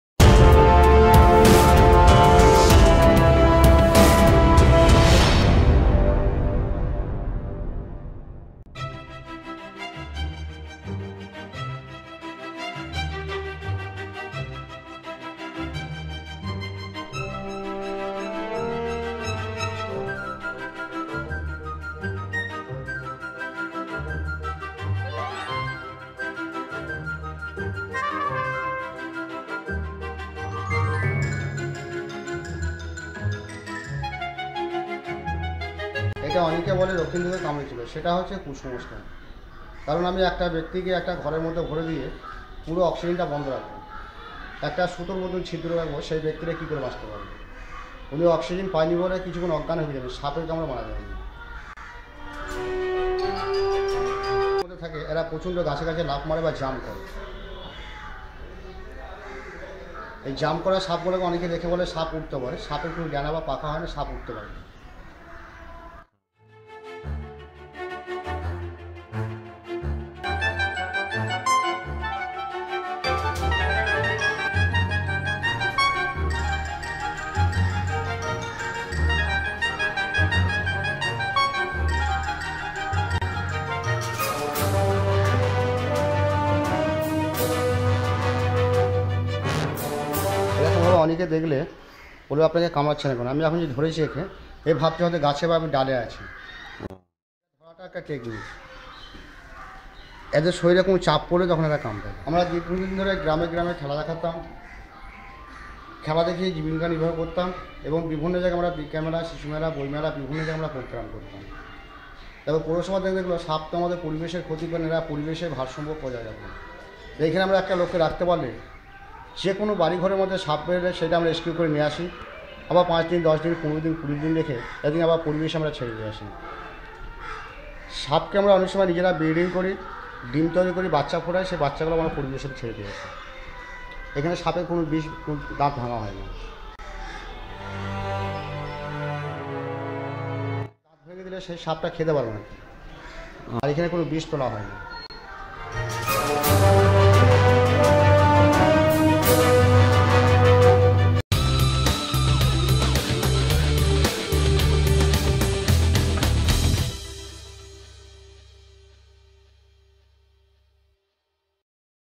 Kal Nagini Snake at Ashoknagar Science Park / Ornate Flying Snake
Kal Nagini Snake Show or Exhibition at Ashoknagar Millennium Science Park .. Chrysopelea ornate is the scientific  name of Kalnagini..It is also called Ornate Flying Snake …This flying snake  is considered mildly venomous. It is not considered dangerous to man. Snakes perform a very important role in ecosystems as predators that help control rodent populations and other pest organisms. We should preserve them with due care for our own survival…Here in this video, a snake-expert of Ashoknagar Millennium Park has expressed his own experience about Kal Nagini Snake..His only message is : Don’t kill the snakes as they are our friends…..Respect his view and please, don’t do it….Now watch the video and don’t forget to thumbs up…